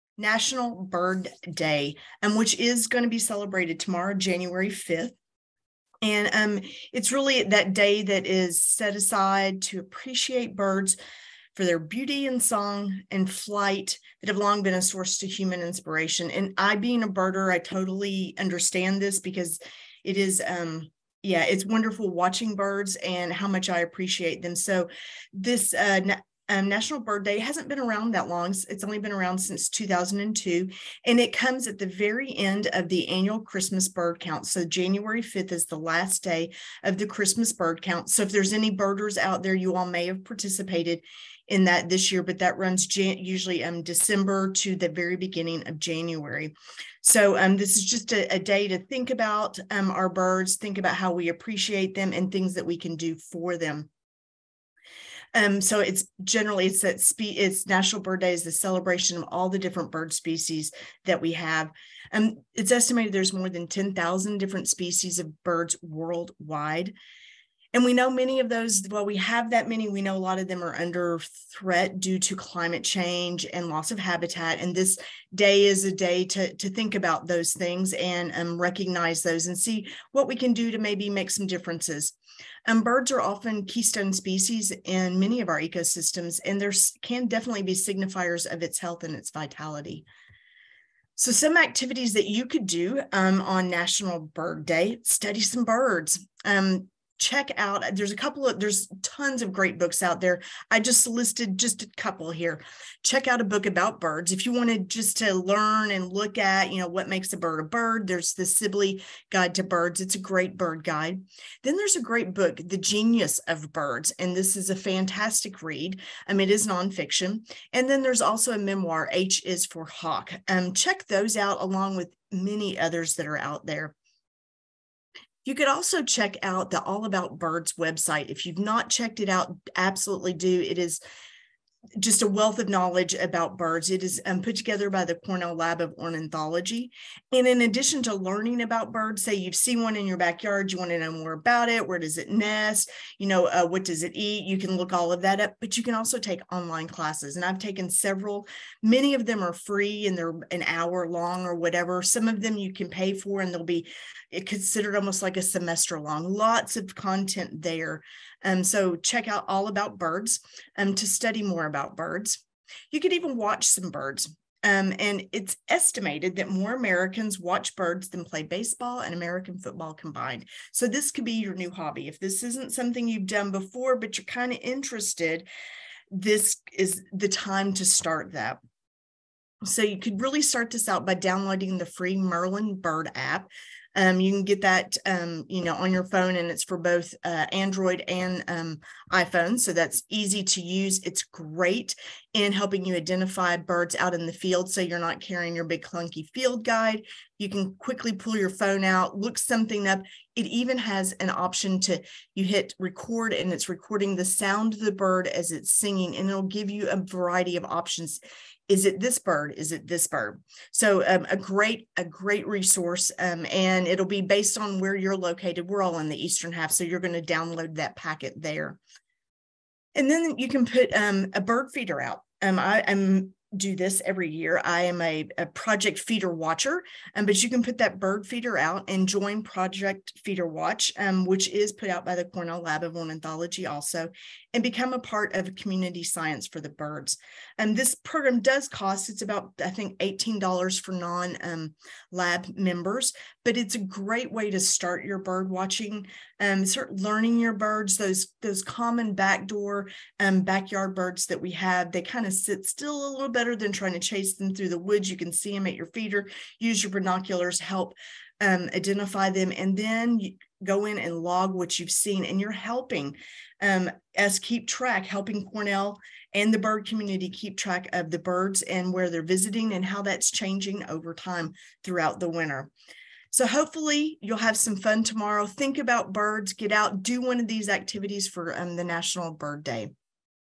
National Bird Day
Celebrate National Bird Day on January 5 with these fun activities~